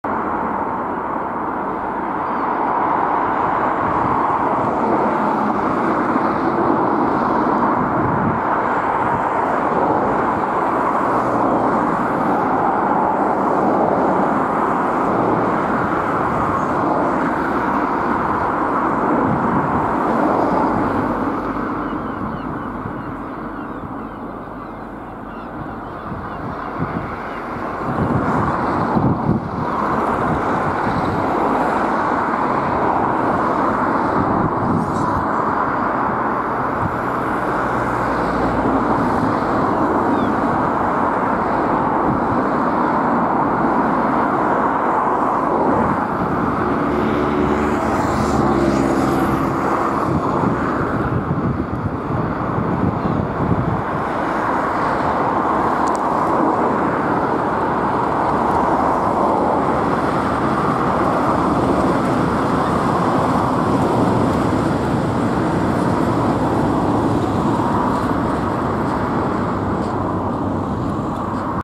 37059 & 37069 pass Gloucester Yard with 6M67 Bridgwater to Crewe. 20/03/2018
37059 & 37069 pass Gloucester Yard with 6M67 Bridgwater to Crewe. 20/03/2018